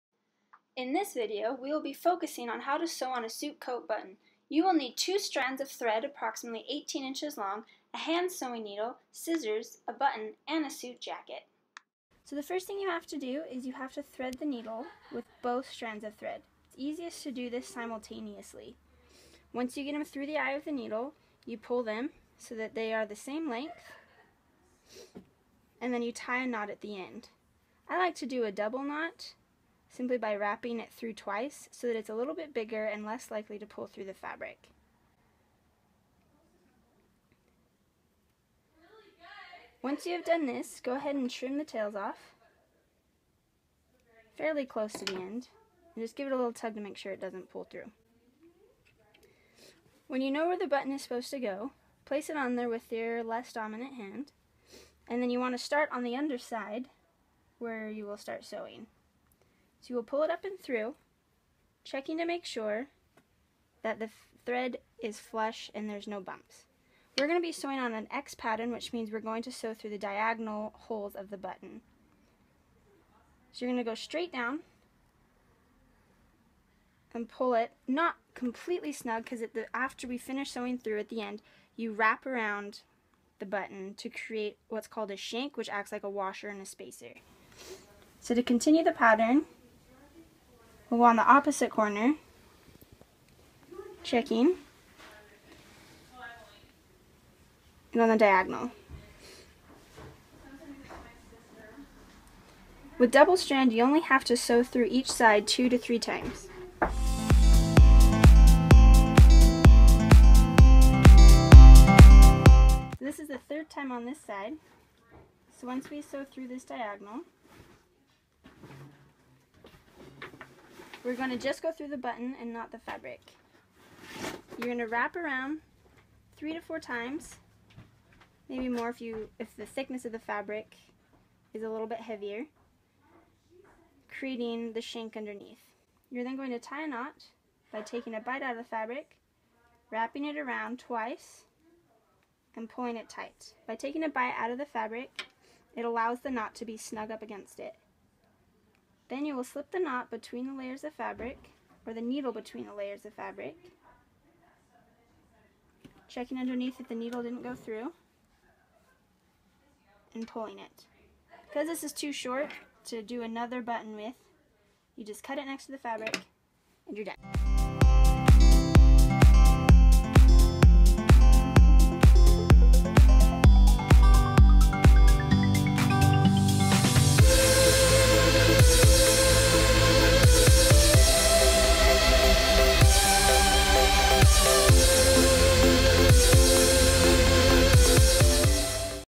DIY Sewing Suit Coat Button
Time to learn with DIY Sewing Suit Coat Button for easy steps. Step by step how to sew suit coat button. It`s fun activity. Enjoy the suit coat button. Let`s learn DIY Sewing Suit Coat Button, let`s see the video